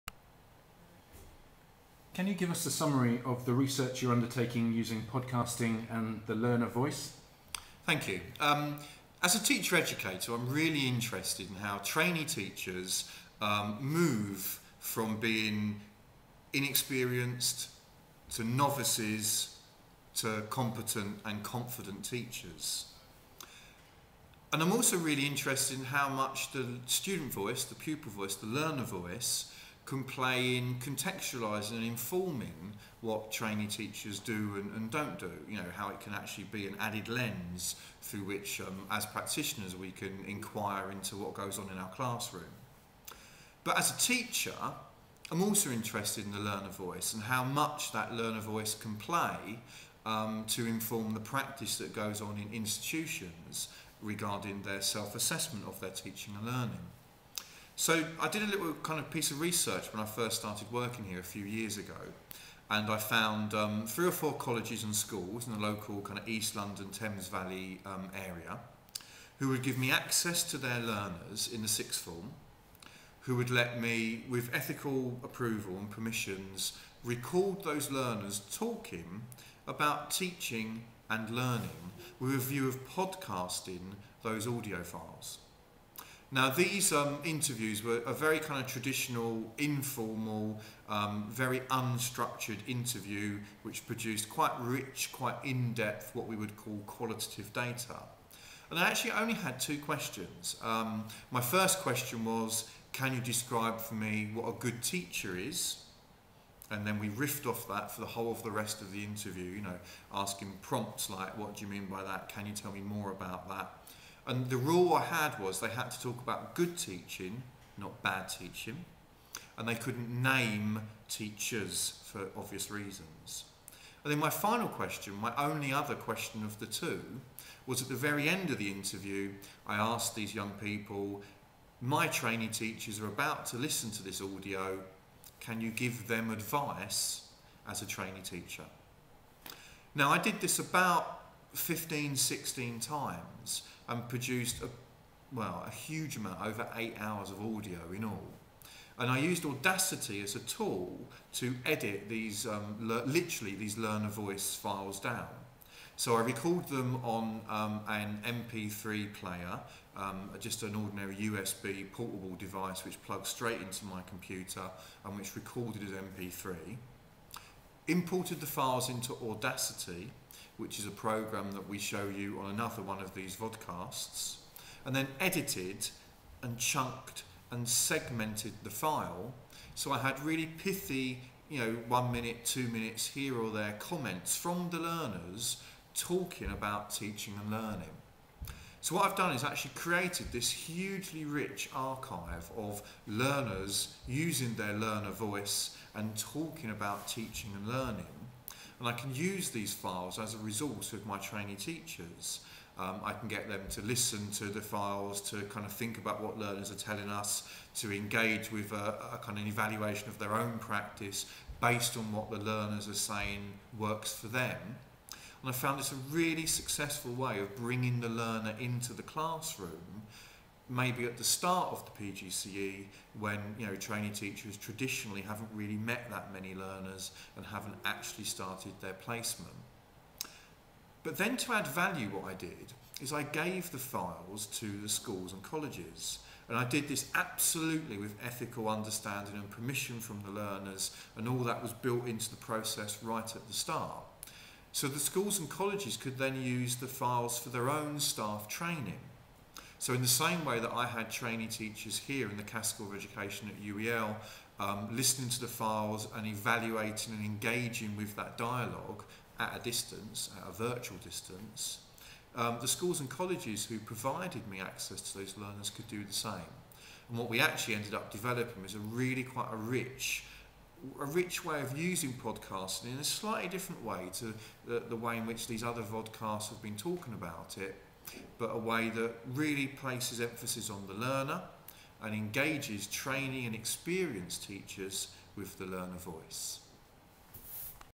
iPodagogy Vodcast #6 - Can you explain your research using podcasting and the learner voice?
In this series of Vodcasts Warren Kidd and Daniel Ayres look at the use of podcasts for teaching and learning. This is the 6th in a series of 7 video interviews we have made looking at podcasting and how it might be used for e-learning, and teaching and learning.